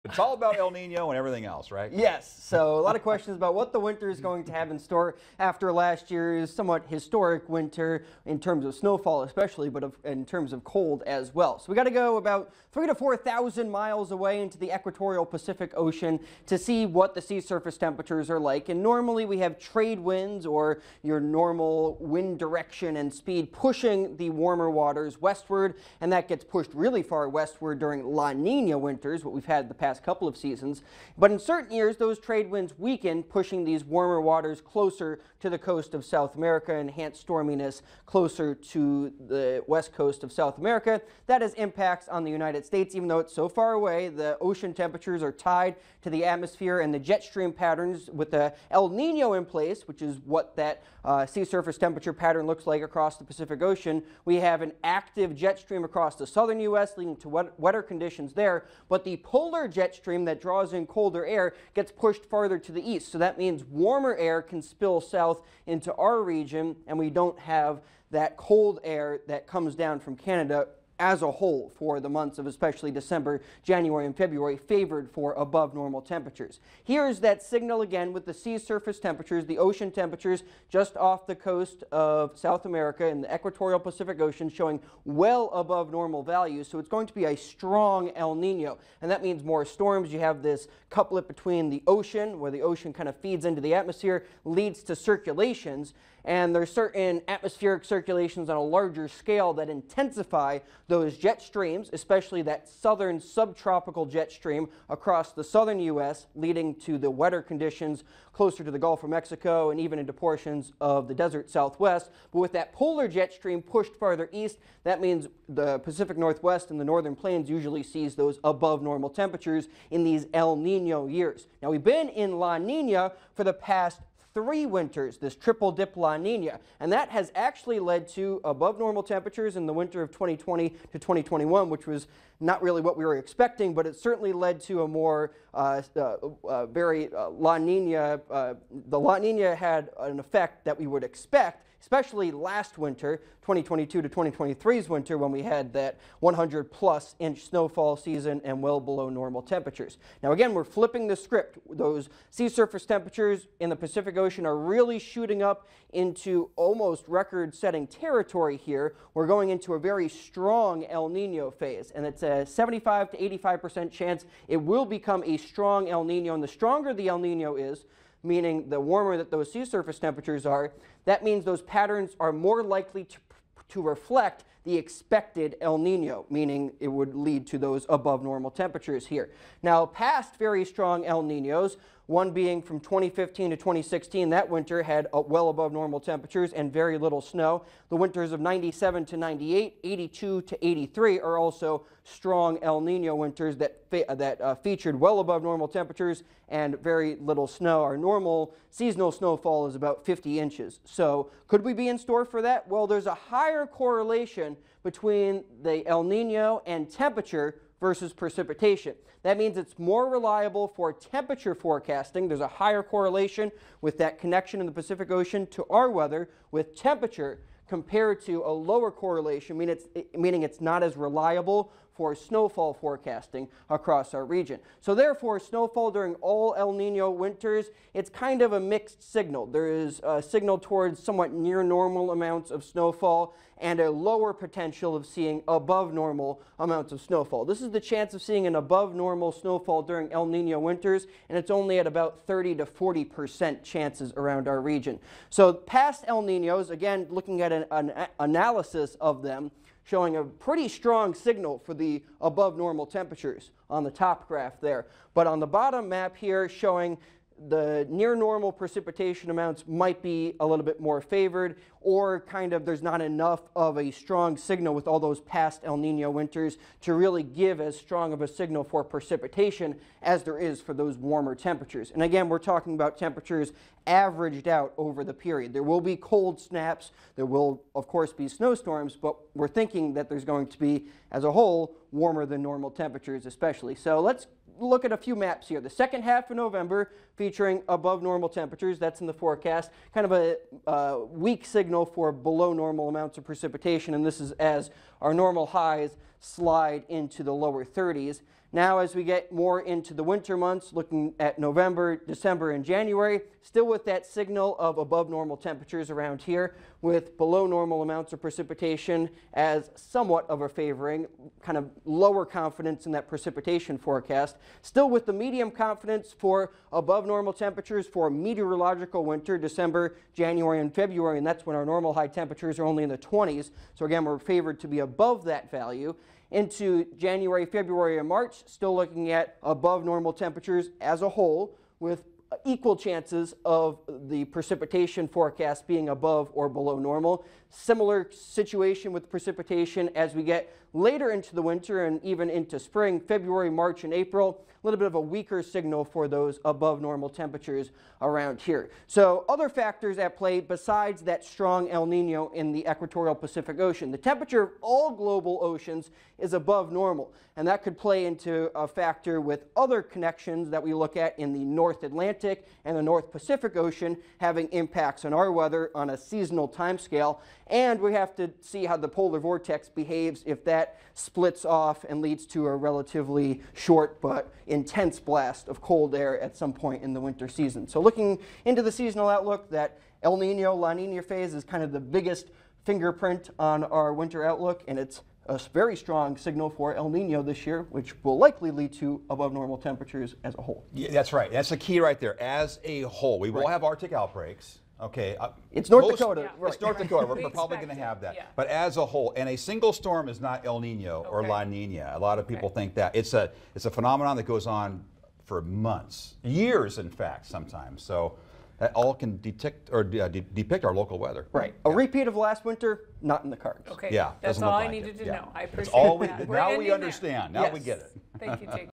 Morse Code of Weather: 2023-24 ND winter outlook: El Niño likely means above normal temps as a wh...
Morse Code of Weather: 2023-24 ND winter outlook: El Niño likely means above normal temps as a whole